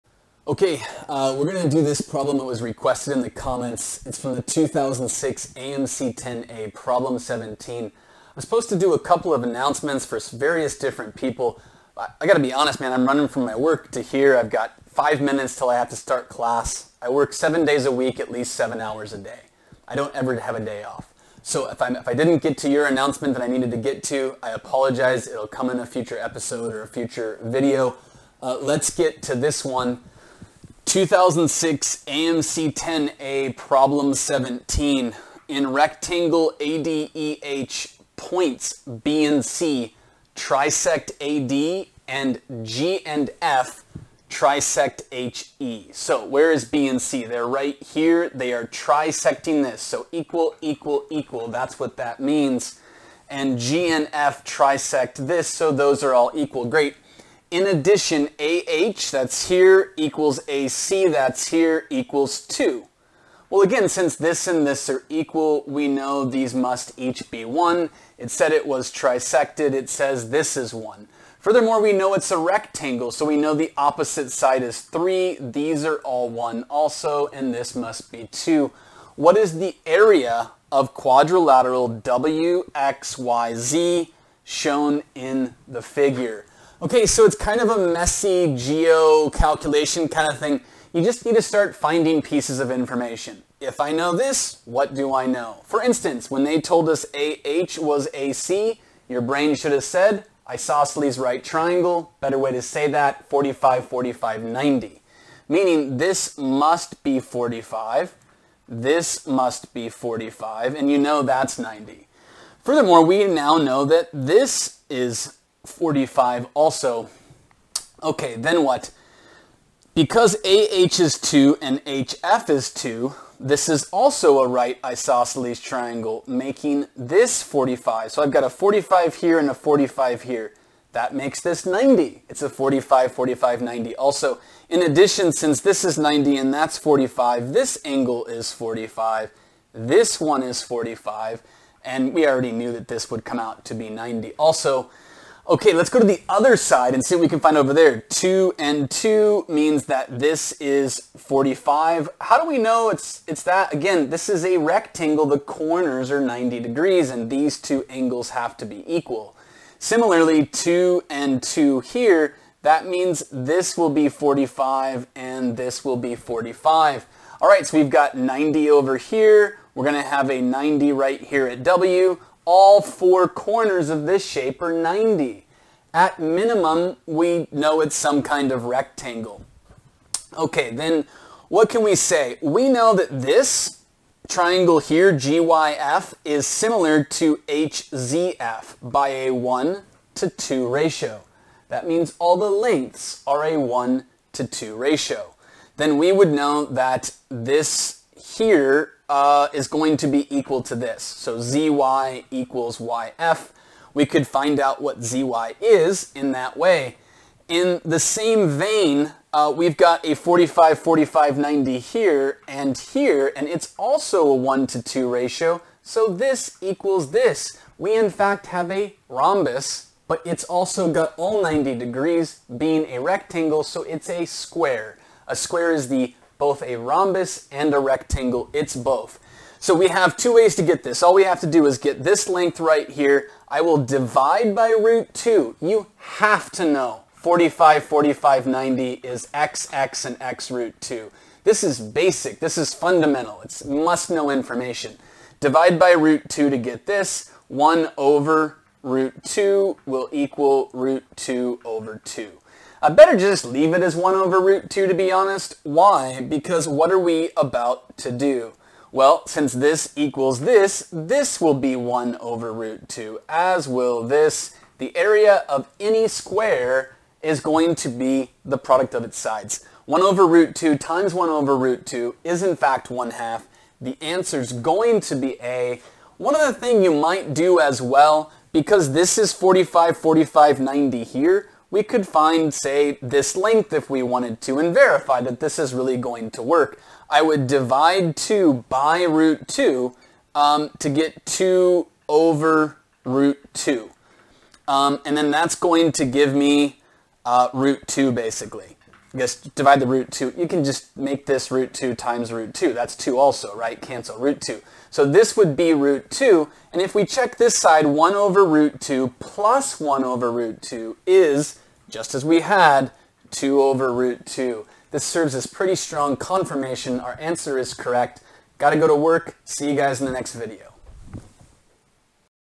2006 AMC 10 A, Problem 17
Strategies and Tactics on the AMC 10. This problem done by request. Mostly attacking the problem by Geometric analysis including the dominant concept of 45-45-90 Triangles.